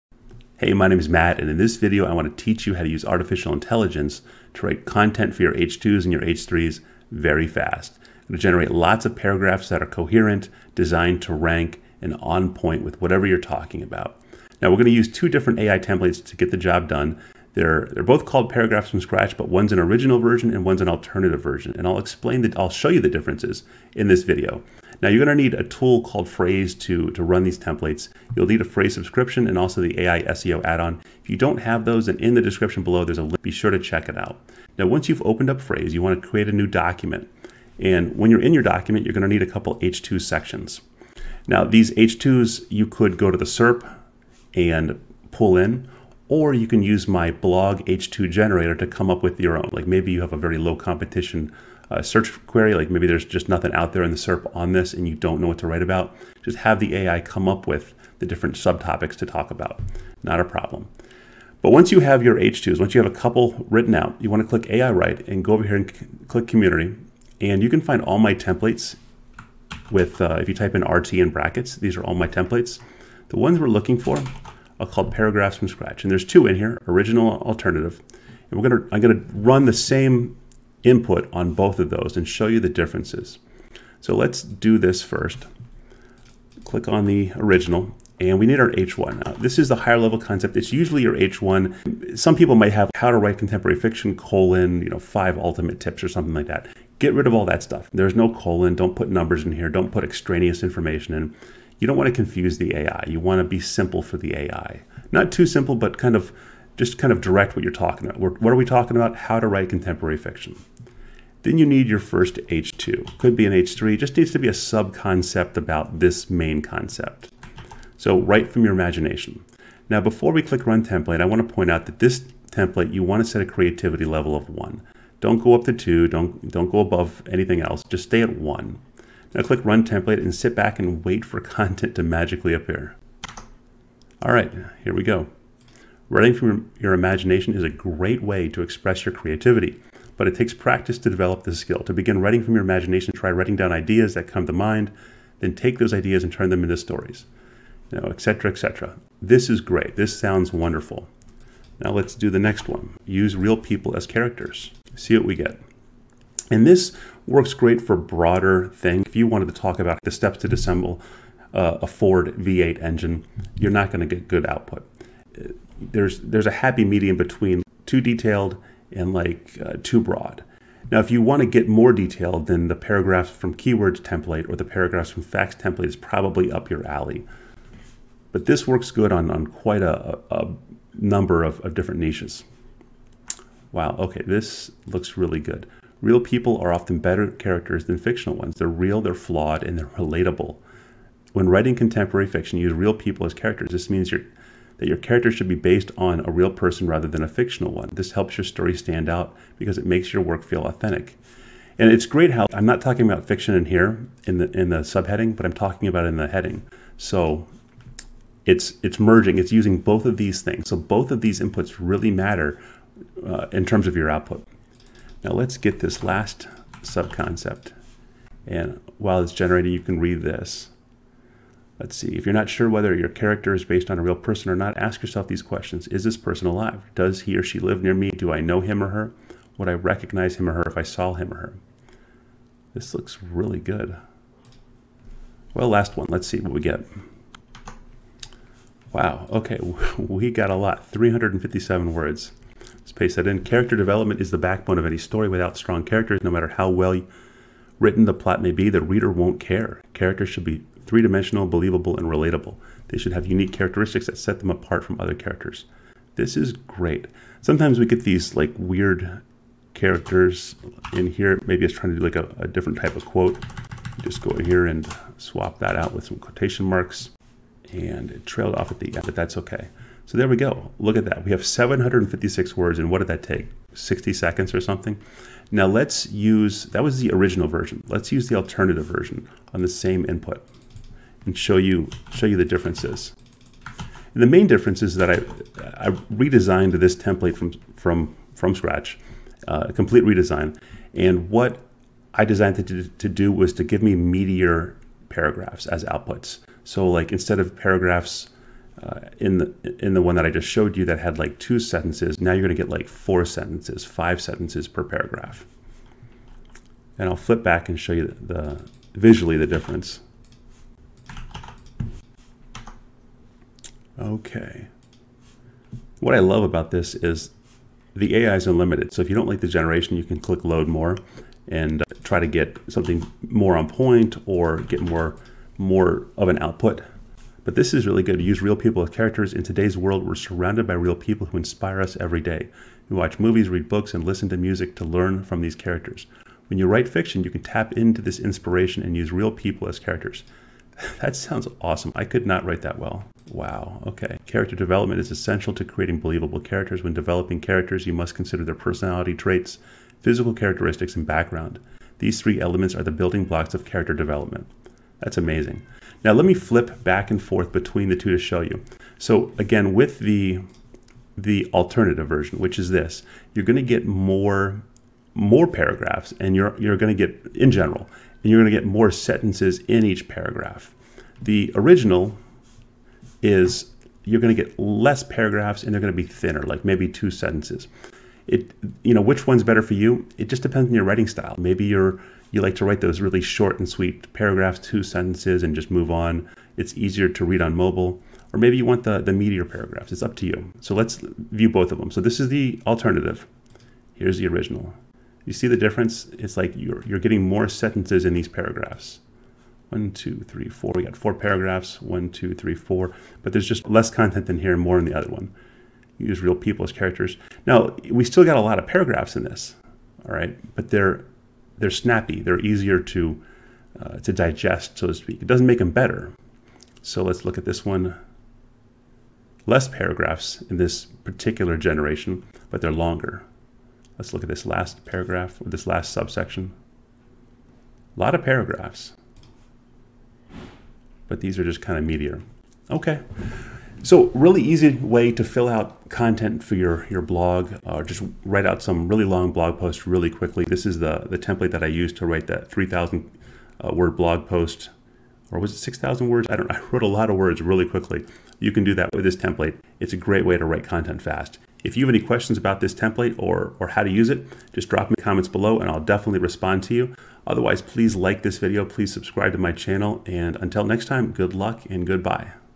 Use Frase AI to Write Paragraphs for Your Blog Sections
Learn how to use my FREE Frase community template "Paragraphs from Scratch" like a pro! Use it to have the AI write TONS of content for your blog post fast.

In this video, I'll explain both templates (the original and the alternative) version. We'll also get a look at how well the templates naturally optimize the output for SEO purposes.